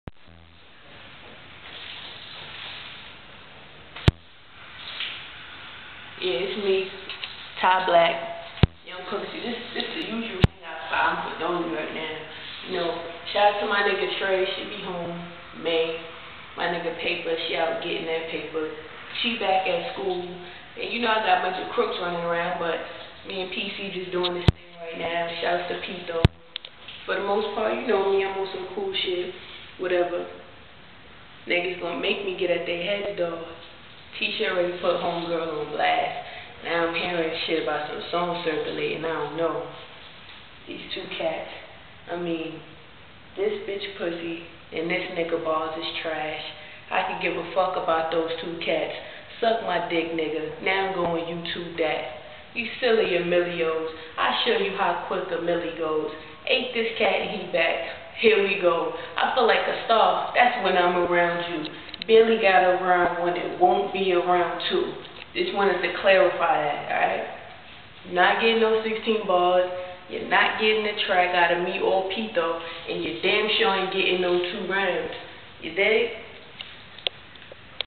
Linden Plaza staircase early 2000’s pt 1
I CLARIFY THE SITUATION ABOUT DUKE I BATTLED AND A AG THATS ON THE MONEY LINE.